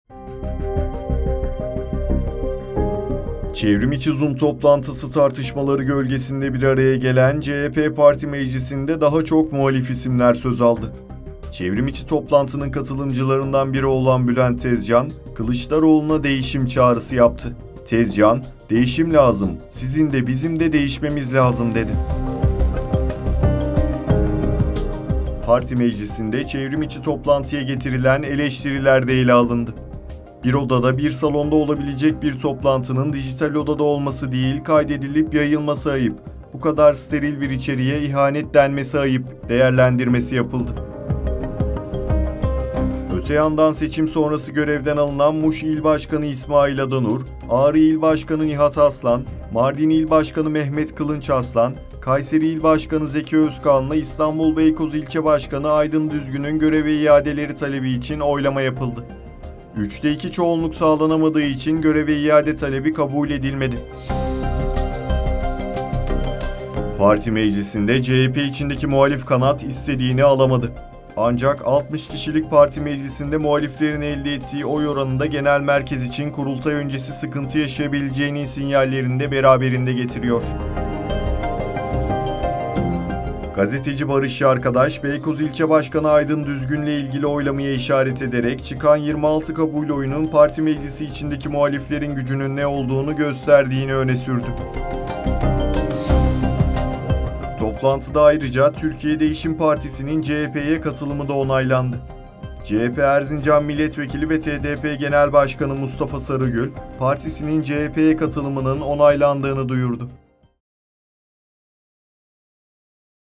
CHP’DE KRİTİK PM’DE NELER YAŞANDI?
CHP PARTİ MECLİSİ TOPLANTISINDA MUHALİF İSİMLER DEĞİŞİM TALEPLERİNİ YİNELEDİ. GENEL BAŞKAN KEMAL KILIÇDAROĞLU BAŞKANLIĞINDA BASINA KAPALI YAPILAN TOPLANTI YAKLAŞIK 12 SAAT SÜRDÜ. MUŞ, AĞRI, MARDİN, KAYSERİ İL VE BEYKOZ İLÇE BAŞKANLARININ GÖREVE İADESİ İÇİN GERÇEKLEŞTİRİLEN GİZLİ OYLAMADA MUHALİFLER İSTEDİĞİNİ ALAMADI ANCAK ORTAYA ÇIKAN TABLO MUHALİFLERİN GÜÇ KAZANDIĞI ŞEKLİNDE YORUMLANDI…